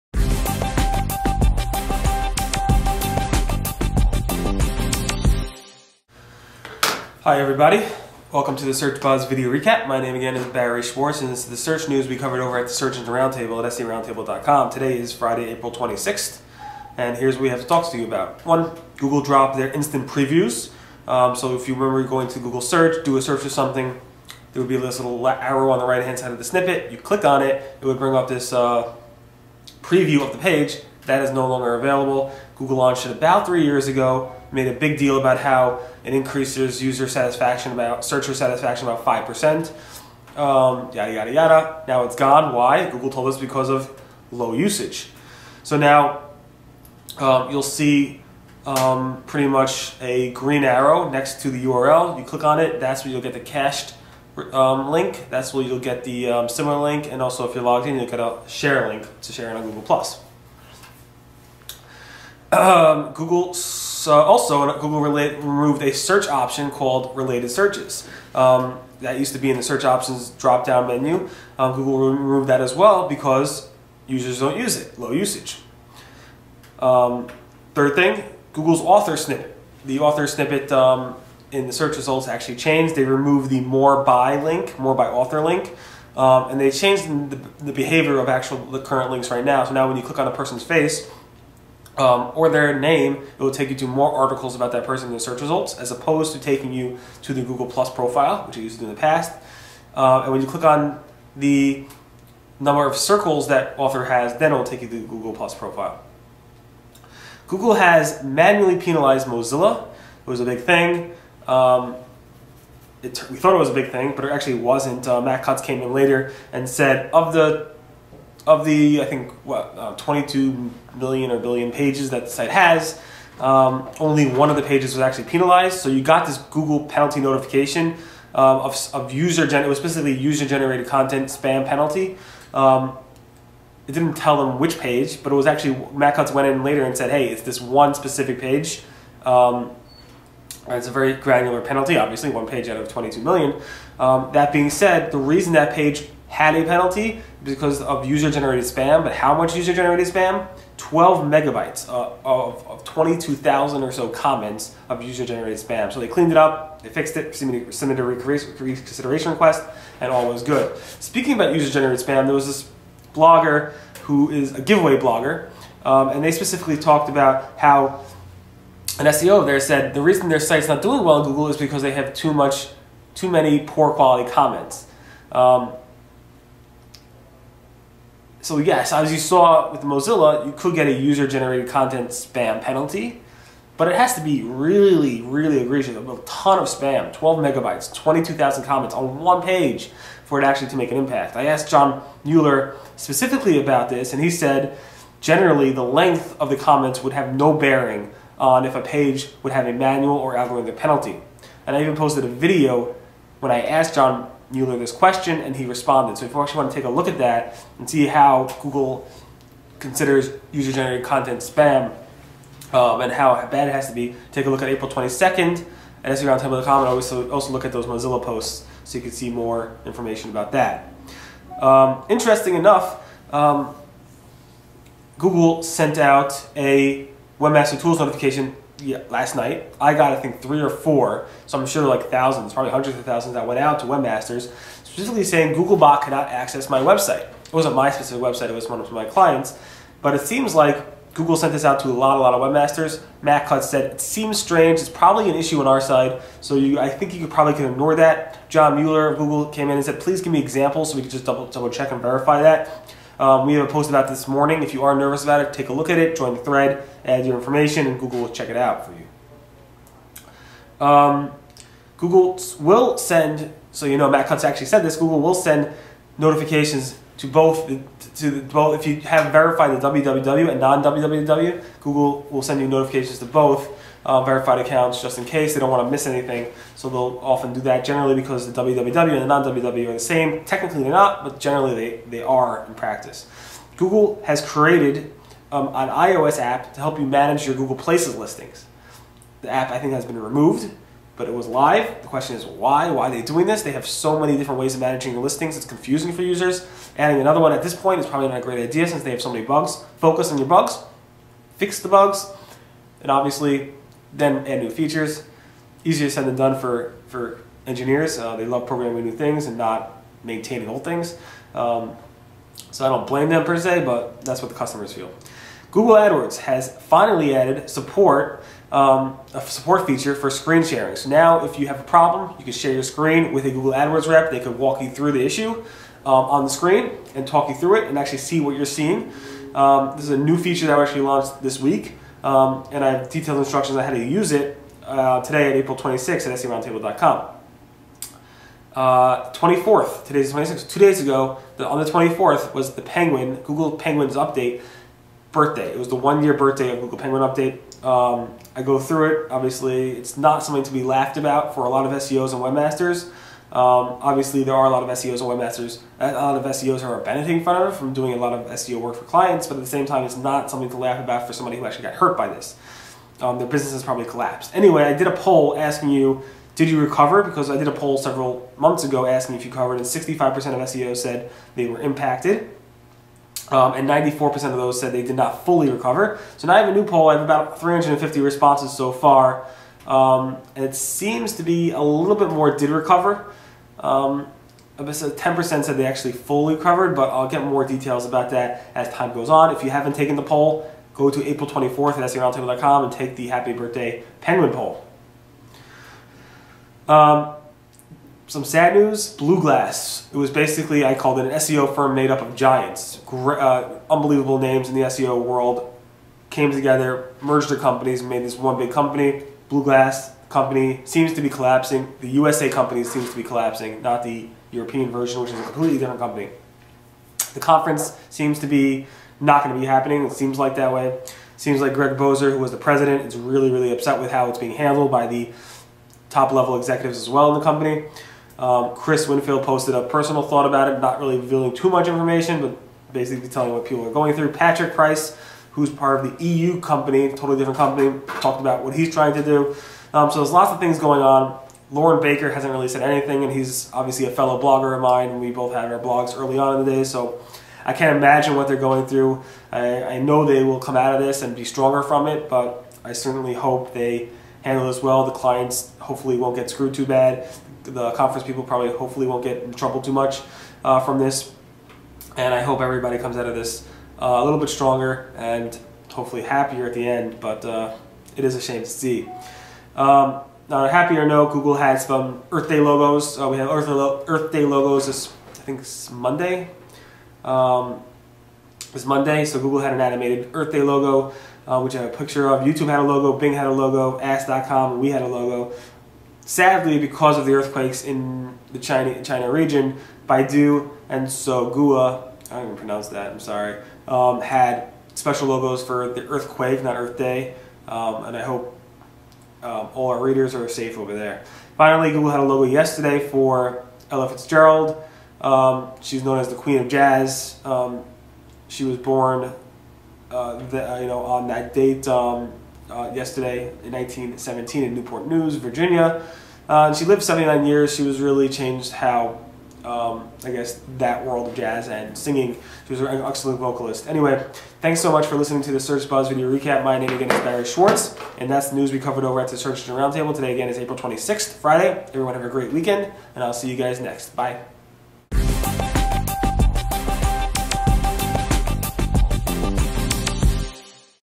Google Removes Previews, Mozilla Penalized, Penguin Turns One & More
- Google made a nice number of changes to the search results, removing instant previews, adding a green arrow next to the URL, dropping related searches and removing the more by author link in the author snippet.  Google also penalized Mozilla for user generated spam content, but it was only on one page and only because it had 12MB of spam.  I then talked a bit more about user generated content spam and how it can impact you.  Google sent out a webmaster tools message that said Googlebot can't access your site, don't worry about it.  Google sends those messages to both www and non-www verified accounts. Google created an iOS app to manage your local listings on Google Maps. AdWords support now has screen sharing.  Google's Penguin update is now one year old.  BlueGlass, the SEO firm made of giants, seems to be toppling down.  We had Earth Day logos and a special logo for the Queen of Jazz.  That was this past week in search news at the Search Engine Roundtable.

Google To Mozilla: You Had 12 Megabytes Of User Generated Spam On One
Ignore That Last Google Webmaster Tools Notification: Googlebot CAN Access Your
Earth Day Logos From Google & Bing While Chinese Search Engines Show Earthquake Logos